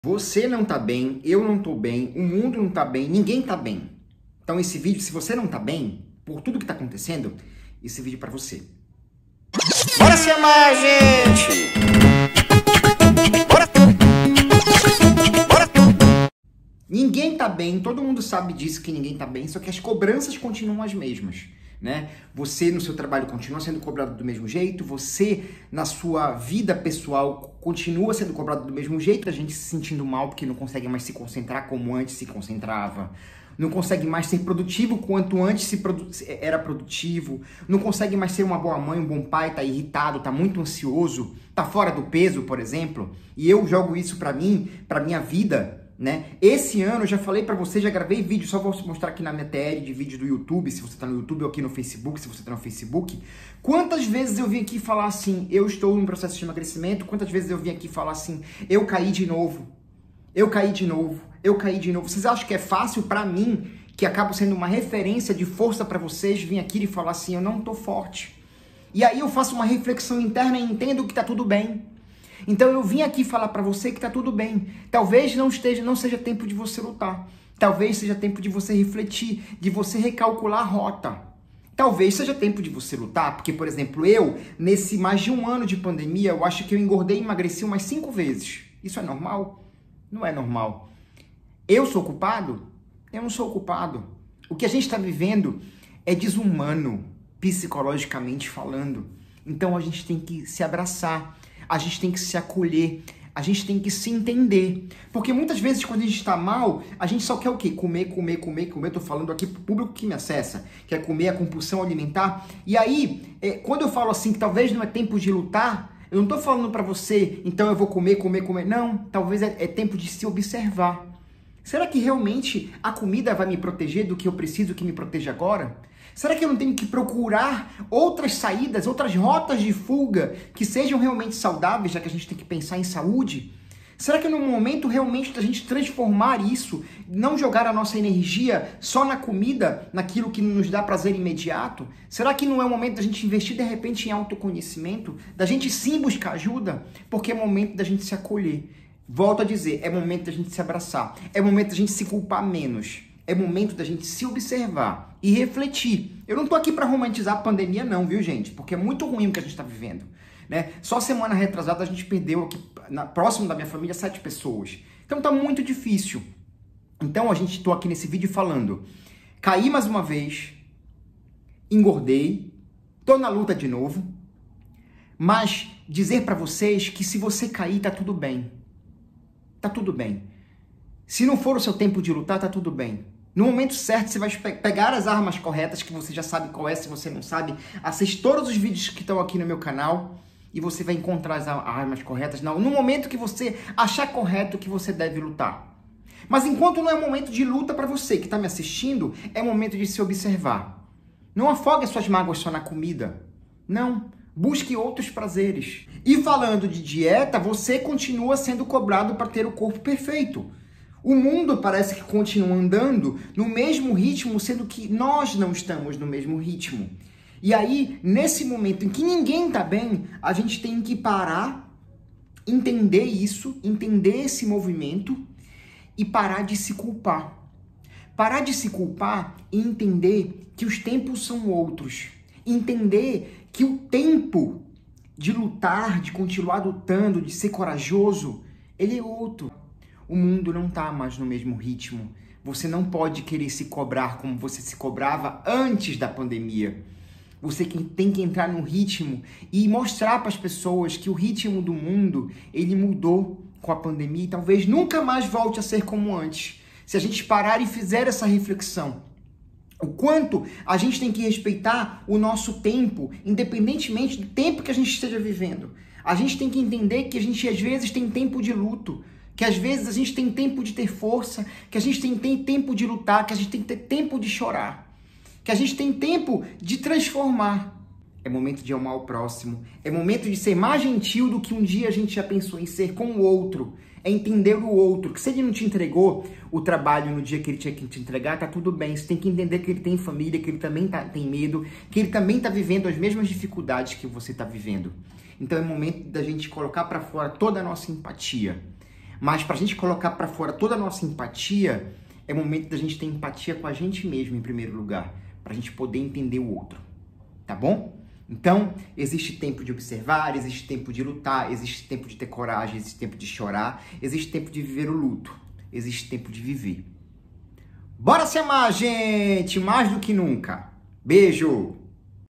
COMO EMAGRECE NO MEIO DESSA PANDEMIA?
Tá todo mundo meio louco e desestabilizado com a pandemia, então o que fazer para conseguir manter o equilíbrio? Dá o play que eu acho que tenho uma ideia!

Siga no Instagram: @eucanseidesergordo